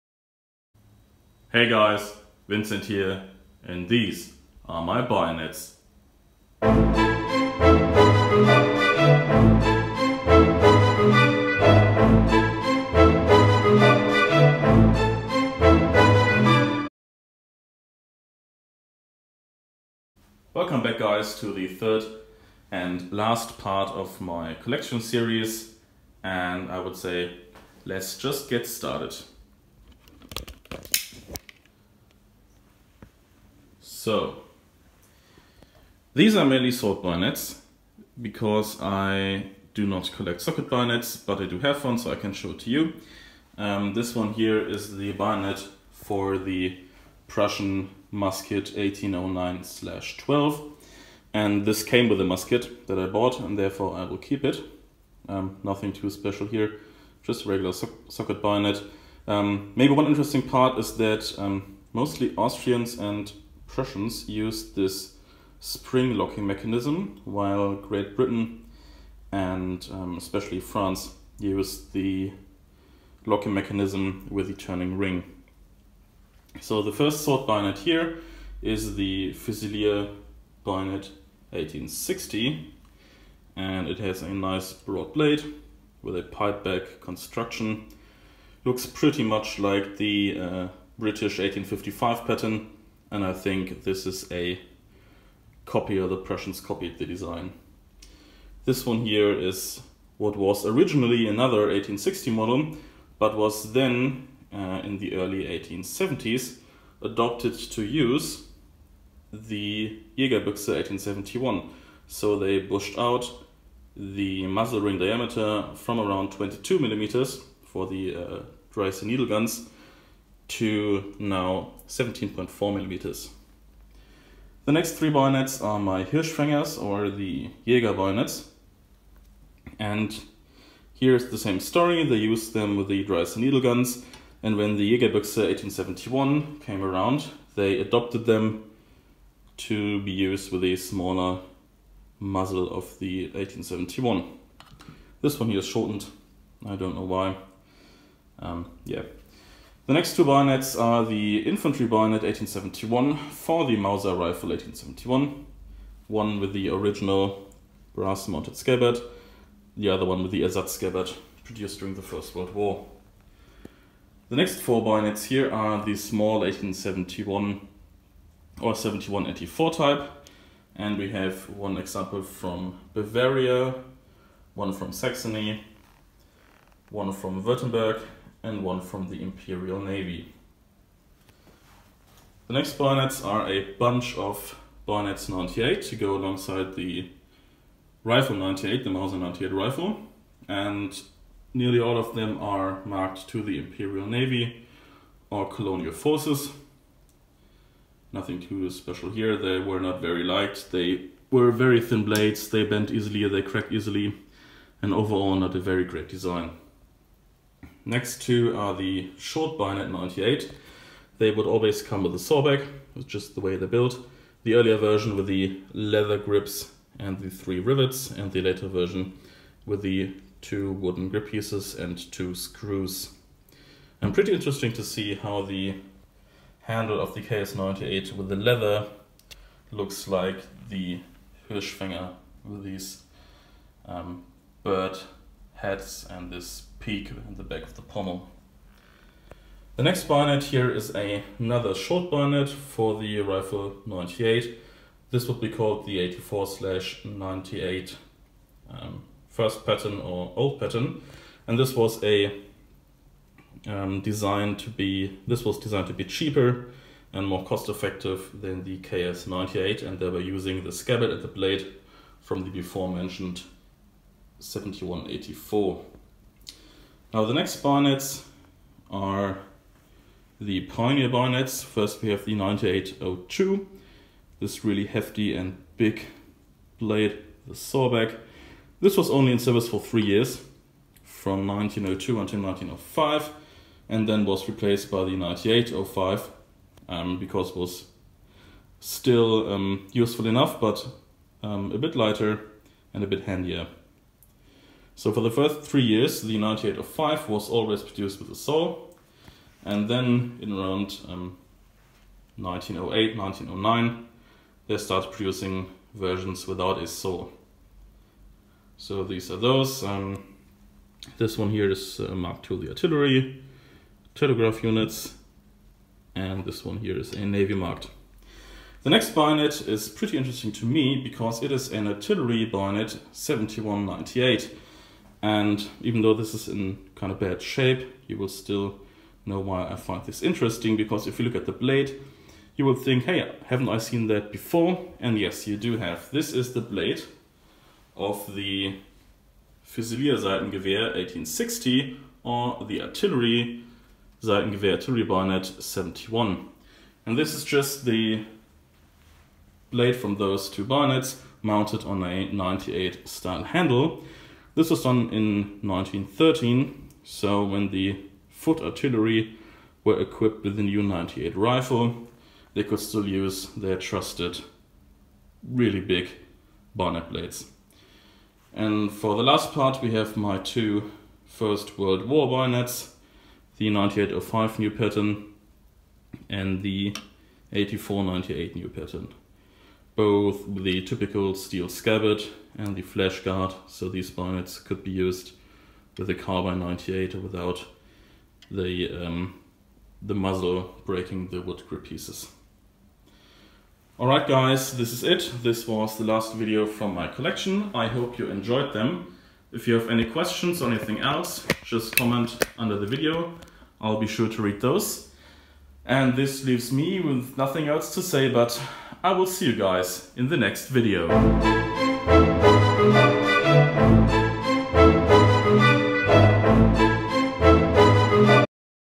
German And Prussian Bayonet Collection, 250 Subscriber Special, 3/3
Have a question about bayonets, sword or rifles?

This is part three of my collection videos.

Part one: rifles
Part two: swords
Part three: bayonets

Vincent